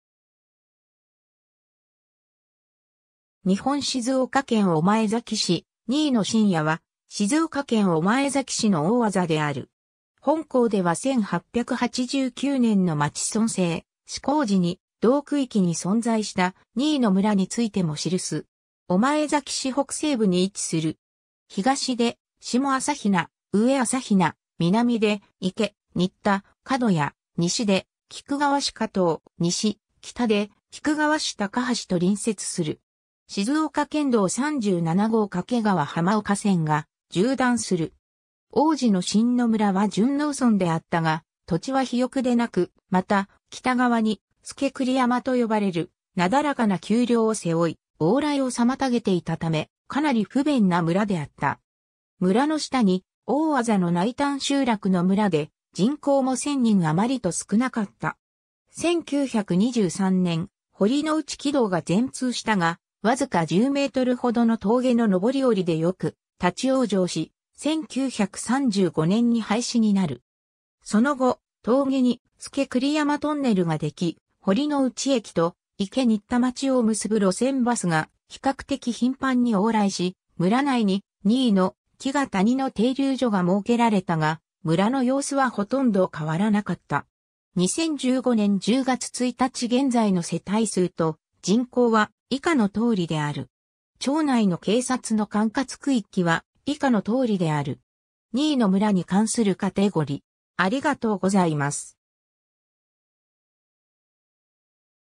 新野 (御前崎市)
御前崎市の町・字
日本 」 静岡県 」 御前崎市 」 新野 (御前崎市) 新野（にいの）は、静岡県御前崎市の大字である。
 本項では1889年（明治22年）の町村制施行時に同区域に存在した新野村（にいのむら）についても記す。
 御前崎市北西部に位置する。
東で下朝比奈・上朝比奈、南で池新田・門屋、西で菊川市河東、西・北で菊川市高橋と隣接する。
静岡県道37号掛川浜岡線が縦断する。
 往時の新野村は純農村であったが、土地は肥沃でなく、また、北側に佐栗山とよばれるなだらかな丘陵を背負い、往来を妨げていたため、かなり不便な村であった。
村の下に大字のない単集落の村で、人口も1,000人あまりと少なかった。
 1923年、堀之内軌道が全通したが、わずか10メートルほどの峠の上り下りでよく立ち往生し、1935年に廃止になる。
そのご、峠に佐栗山トンネルができ、堀之内駅（現菊川駅）と池新田町を結ぶ路線バスが比較的頻繁に往来し、村内に、新野・木ヶ谷の停留所が設けられたが、村の様子はほとんど変わらなかった。
 2015年（平成27年）10月1日現在の世帯数と人口は以下の通りである。
 新野村 (静岡県)に関するカテゴリ: